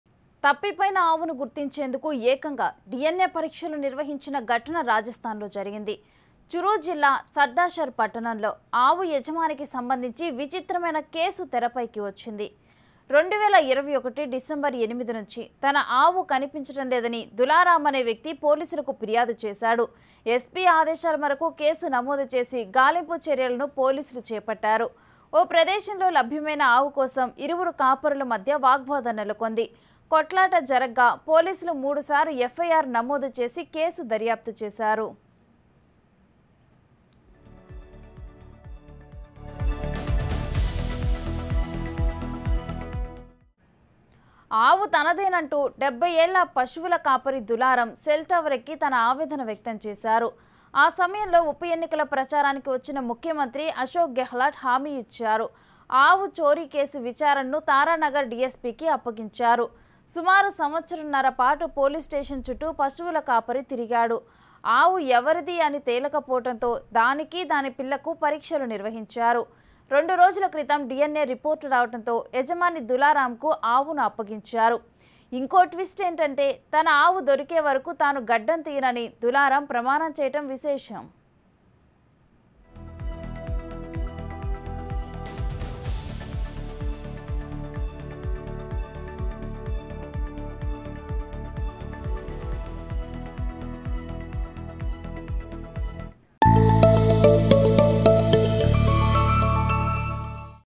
Cow DNA Test | ఆవుకు డీఎన్ఏ పరీక్షలు నిర్వహించిన పోలీసులు..! | BIG TV Telugu
Cow DNA Test | ఆవుకు డీఎన్ఏ పరీక్షలు నిర్వహించిన పోలీసులు..! | BIG TV Telugu News Channel | @BIGTVLive
Cow DNA Test